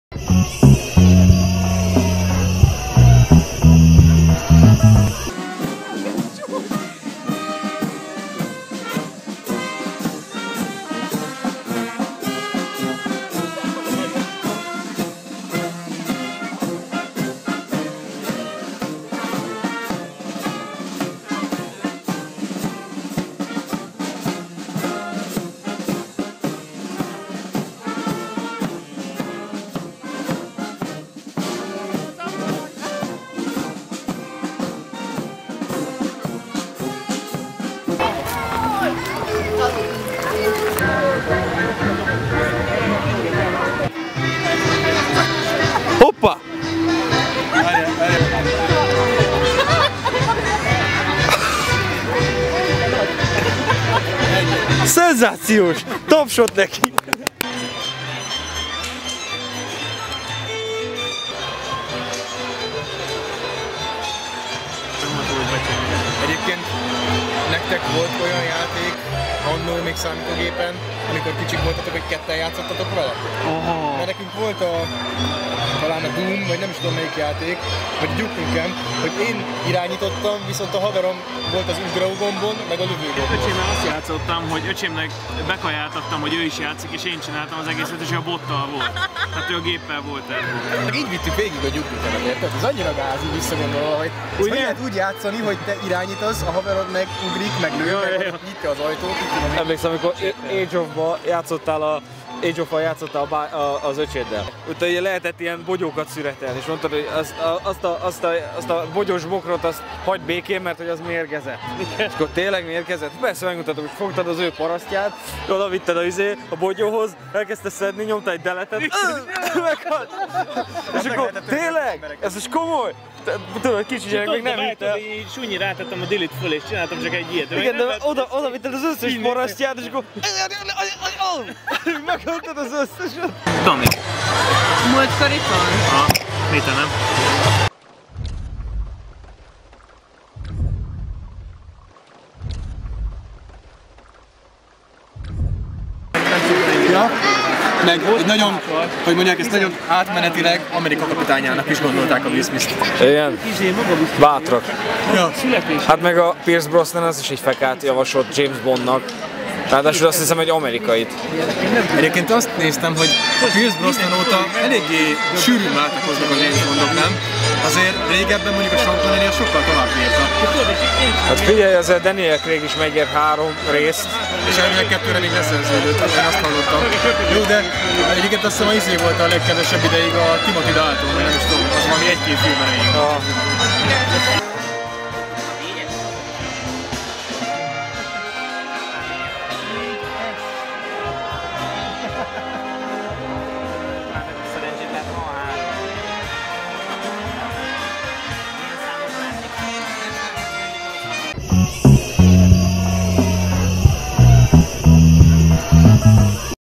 Sor-Szür-Feszt 2013
*Húsvéti tojat No. 1): "Sörpad borogatás"
*Húsvéti tojat No. 2): "Ágyú"
(Görgess lejjebb a megoldásokért)

Q: Melyik a kedvenc fesztiválod?
(a válaszokat "A:" kezdéssel jelöljétek meg kommentben)

A banda egyes tagjai meglátogatták a Soroksári Szüreti Fesztivált, amely évente egyszer szórakozásunk színhelye. Sok ismerőssel találkozhatunk, hagyományőrző zenék és finom borok teszik jobbá a hangulatot, de mi akkor is a saját társaságunkért megyünk le.

* 2 Húsvéti tojat: MEGOLDÁSOK
*Húsvéti tojat No. 1): (Sörpad borogatás!) 00:58 Dani lehajol, és mögötte feltűnik egy felborított sörpad. Nyilván túl jó volt már ilyenkor is a fesztiválhangulat.
*Húsvéti tojat No. 2): (Ágyú) 02:13 A műanyag sörös pohár mögött a kamera fedelén jól látható a márkajelzés: Canon (tudjuk, hogy nem így kell írni).